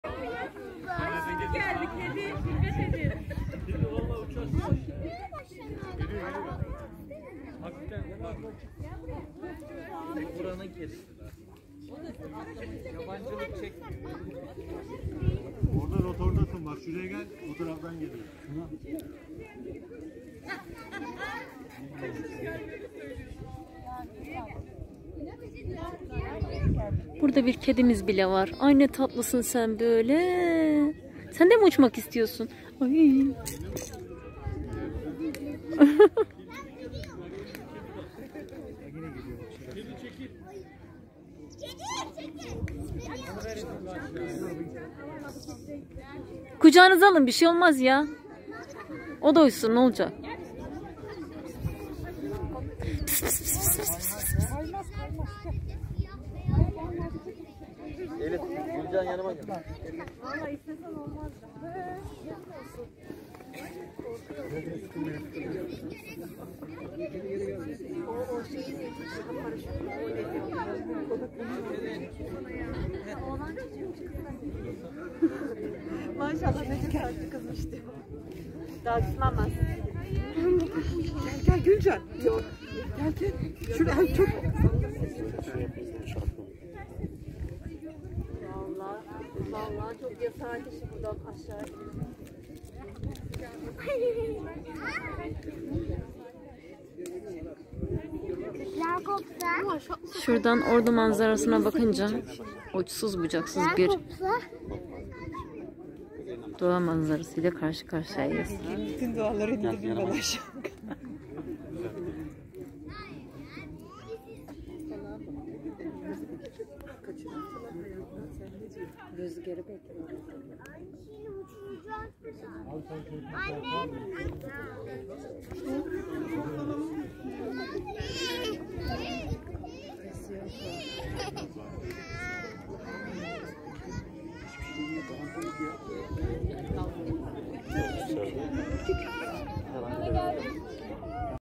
Ordu Boztepe'de yamaç paraşütü gerçekleştirmek isteyen gençlerin, rüzgarla imtihanı...